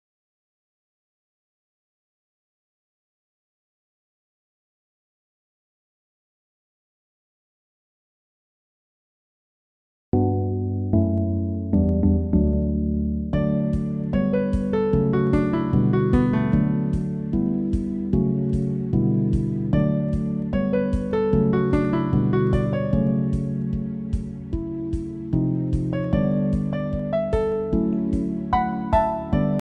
WonderingDrift & All other GM81 Games Work Under WINE Now?
I didn't do anything it kinda just started working.
i guess WINE just got an update and now GM81 Games work
anyway this is great now i can finally make good progress on the game.